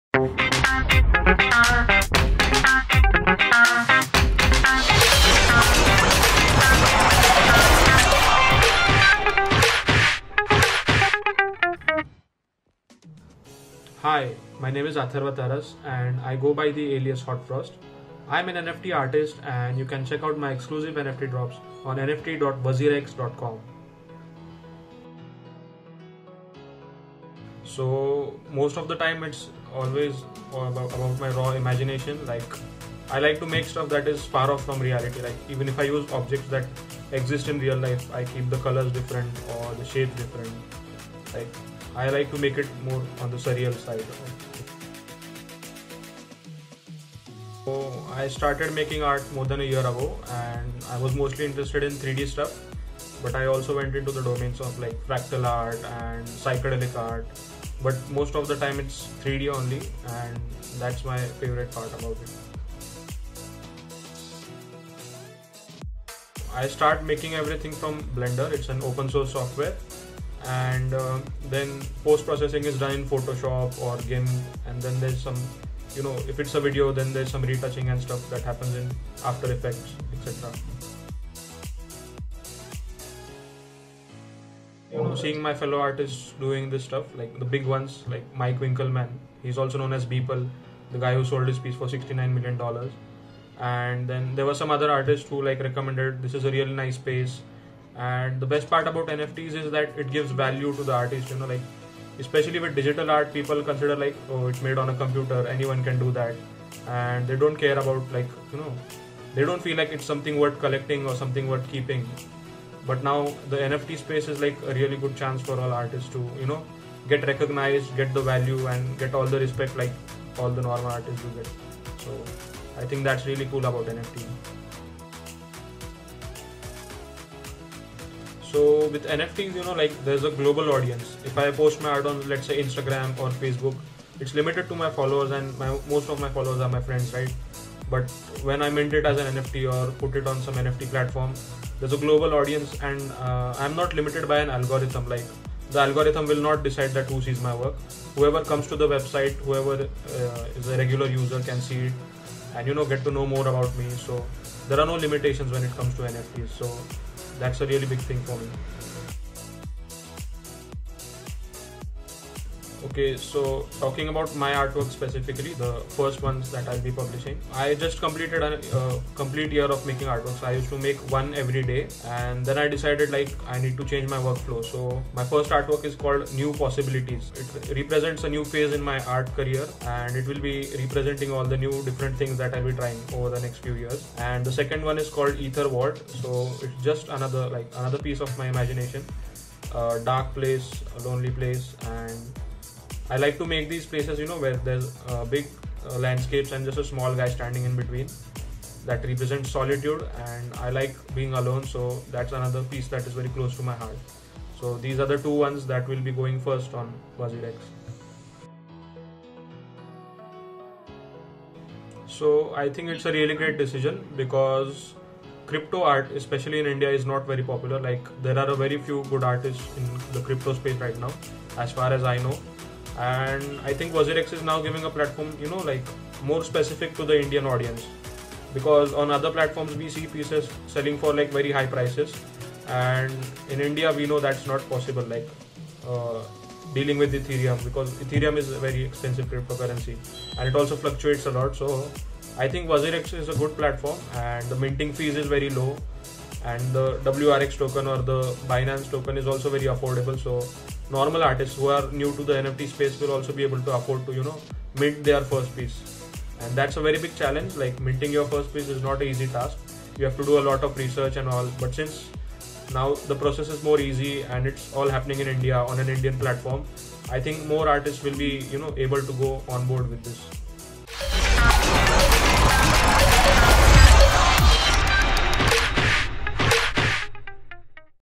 WazirXNFT Marketplace | Creator's Spotlight with Atharva Taras
Atharva is a multidisciplinary CG Artist based out of Pune, India. He makes Abstract, Surreal 3D art in Blender. He aims to express thoughts and emotions through his art.